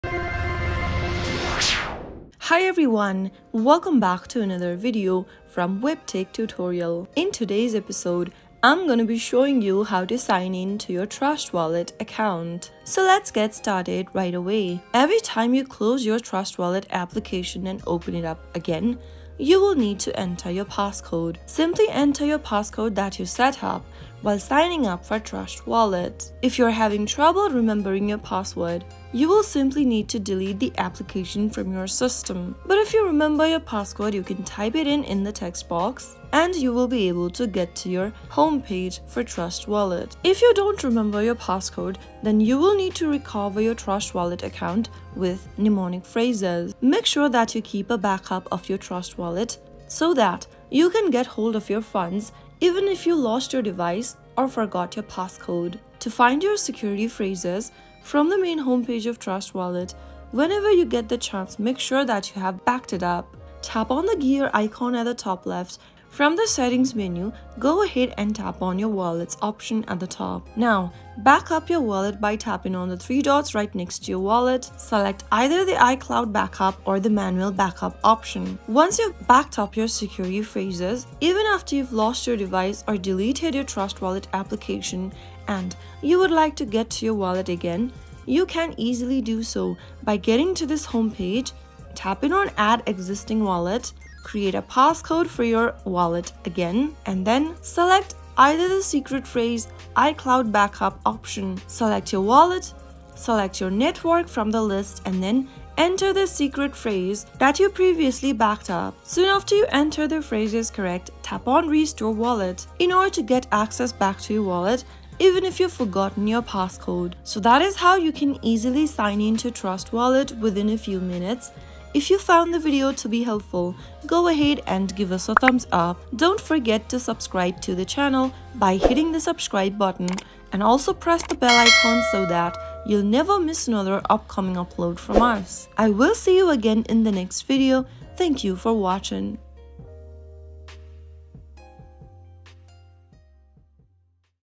How to Login Sign In Trust Wallet Account Online? Trust Wallet Login 2024
Unlock the power of your cryptocurrency holdings with our user-friendly guide on signing in to your Trust Wallet account. Whether you're a new user or looking for a quick refresher, watch now for hassle-free access to your digital assets.